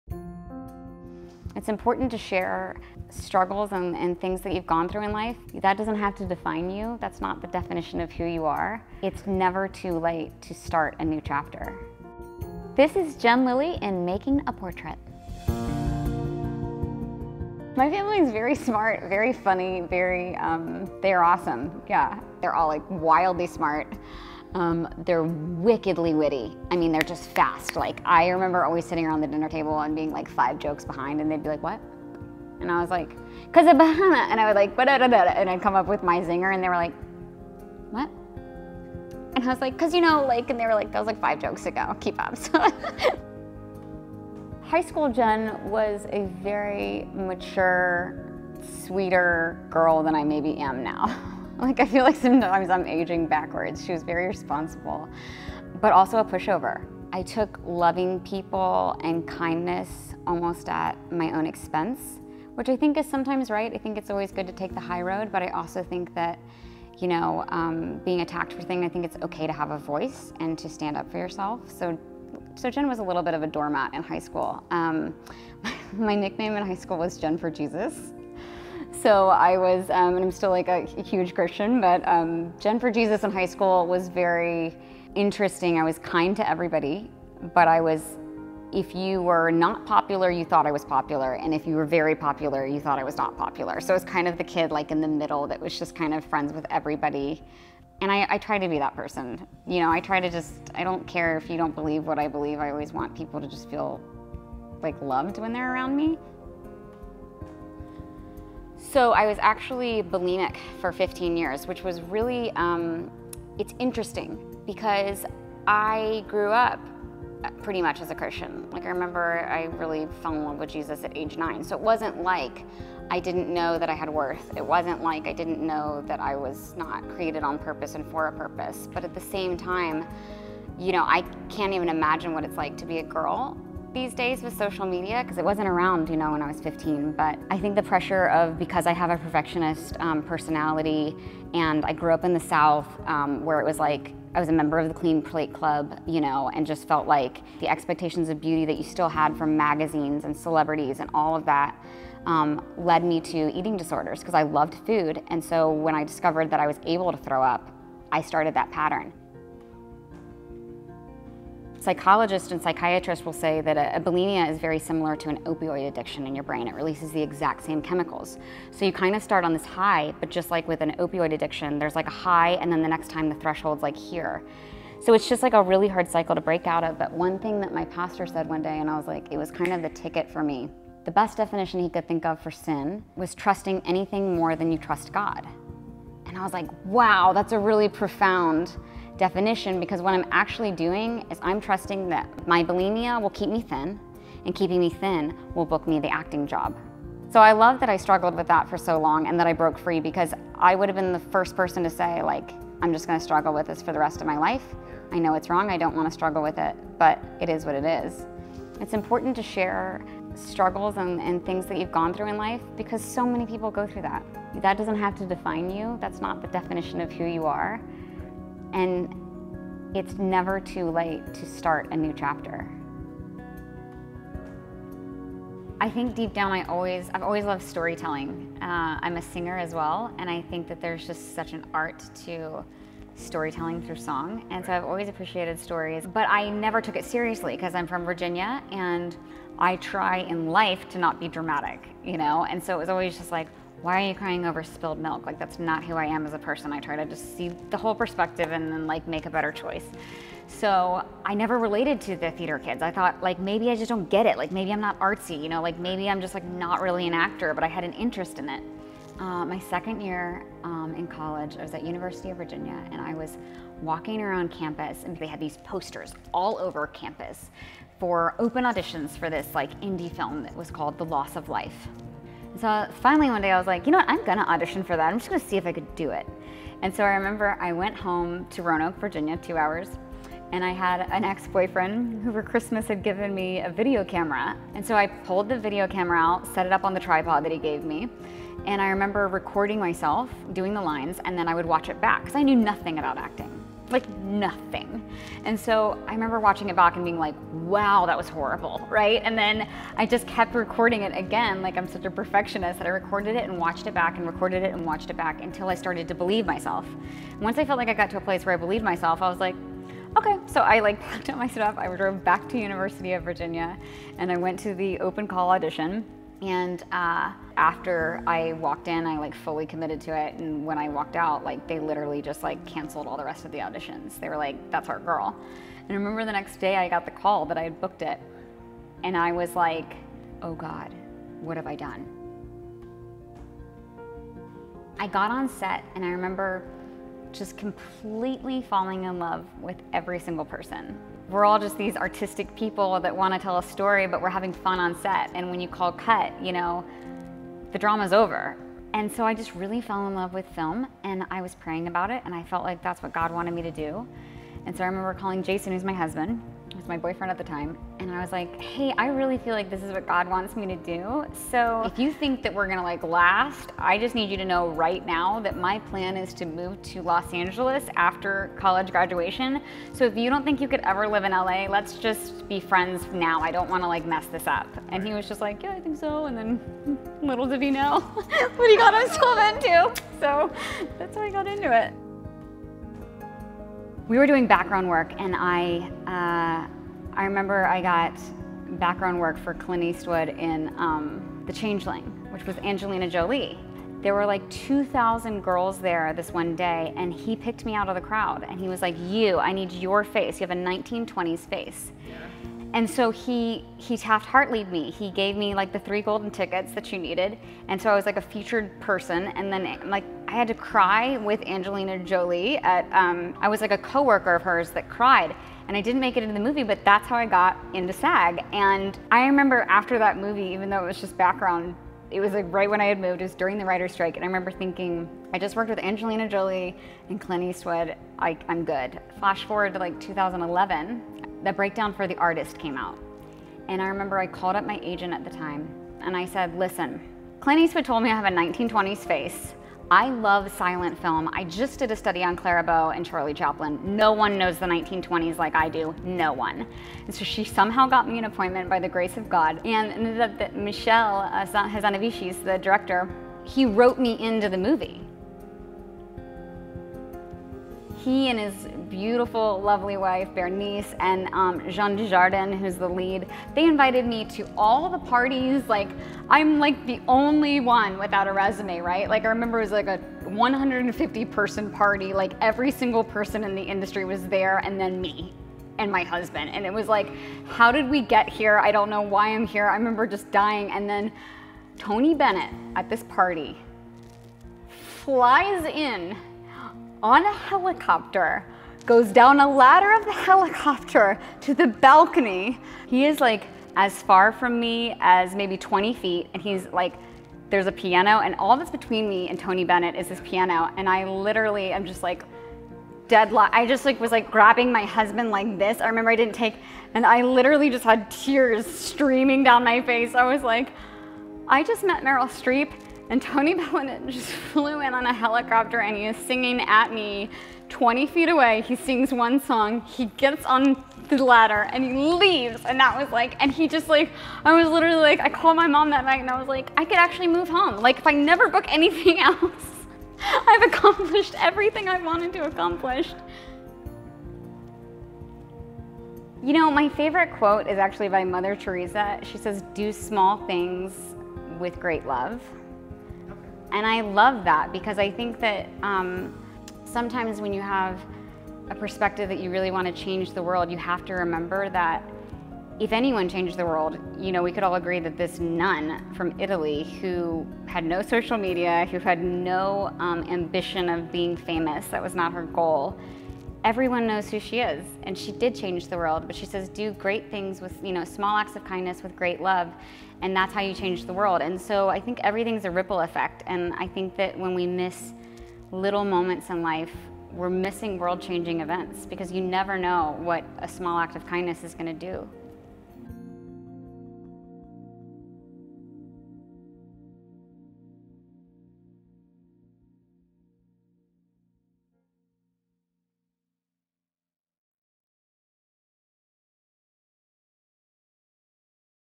Overcoming My Downfalls Through Faith | Making a Portrait - Jen Lilley
Jen Lilley, star of NBC's Days of Our Lives, opens up about her lessons learned from being a pushover in high school, and the strength she found to overcome her addiction to bulimia. Through her strong Christian faith, and love of storytelling, Jen was able to push through and work towards her pursuit of performing in front of the camera.

Directed & Portraits:
Isaac Alvarez

Director of Photography:
Rafal Cwiok

Camera & Sound Engineer:
John Tieng

Producer:
Alfonso Partida

Edited by
Rafal Cwiok